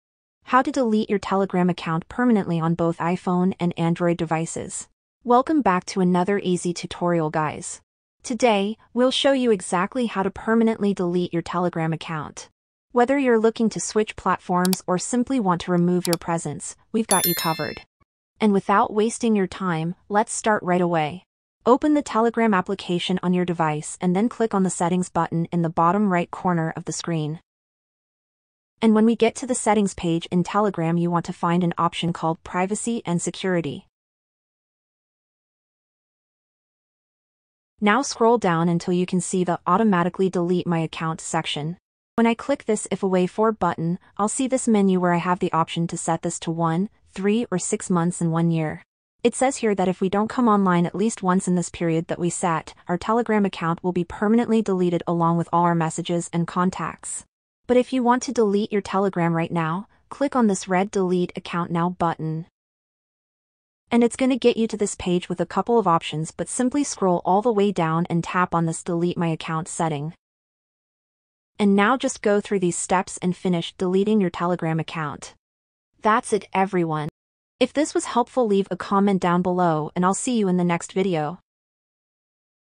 How to Delete Telegram Account Permanently (2023)
Are you looking to say farewell to your Telegram account for good? Whether you've found a new messaging platform or simply want to take a break from the digital world, we've got you covered! In this comprehensive guide, we'll walk you through the exact steps on how to delete your Telegram account permanently.